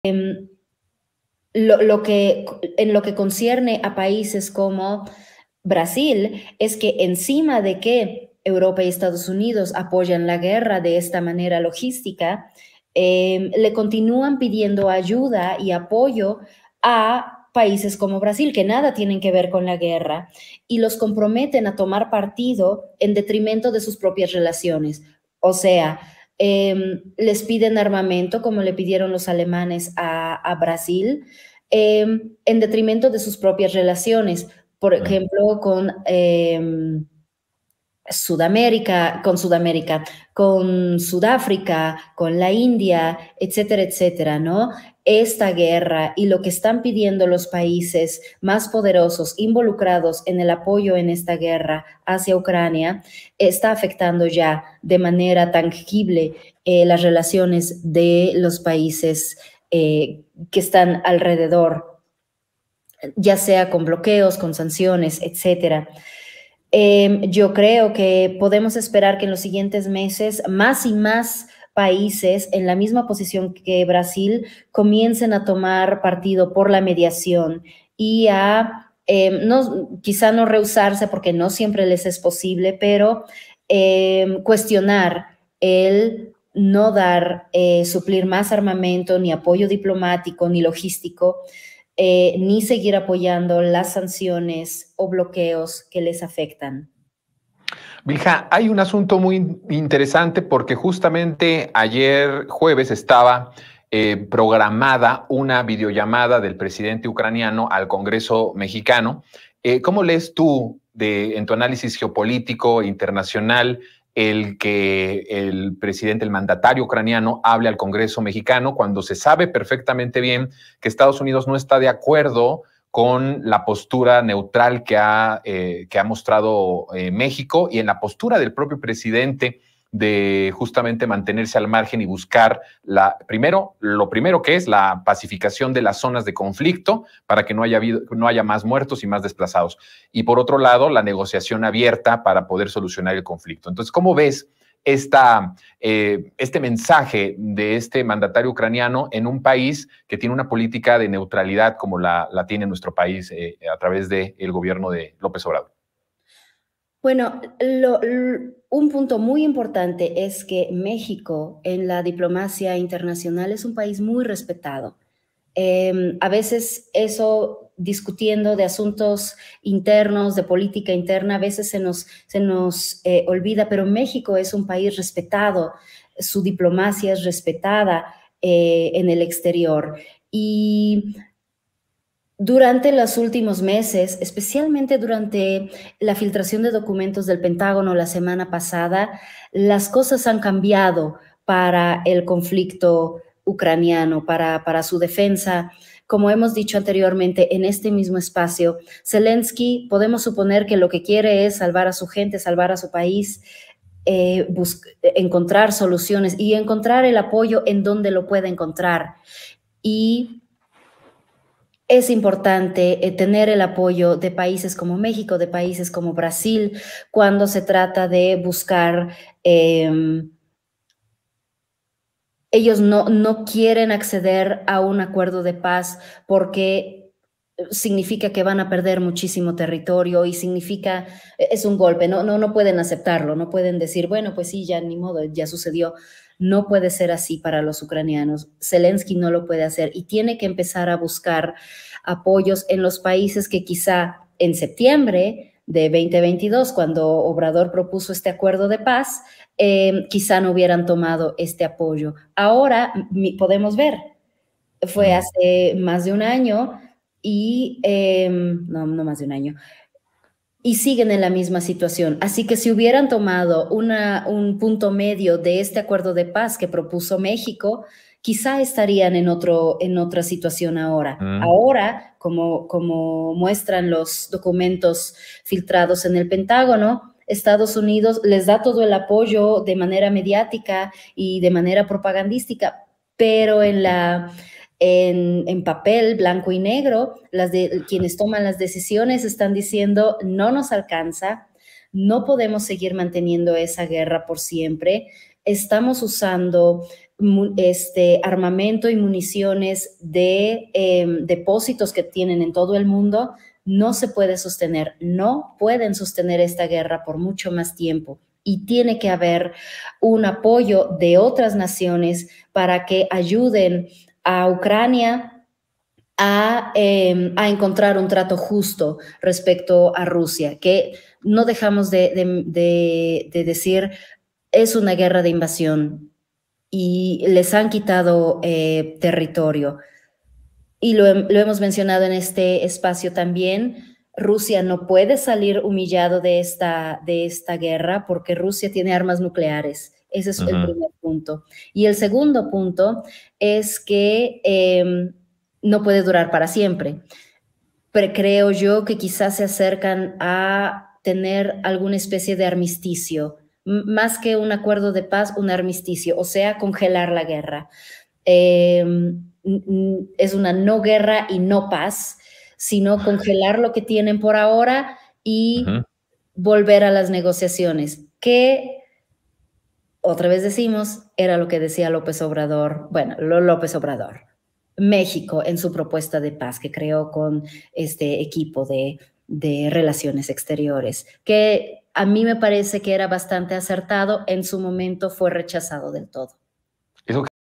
MÉXICO EN LA DIPLOMACIA INTERNACIONAL ES UN PAÍS MUY RESPETADO: BILHA CALDERÓN
El periodista Jonathan Pérez Charlando con Bilha Calderón abordan el tema de que México en la diplomacia internacional es un país muy respetado.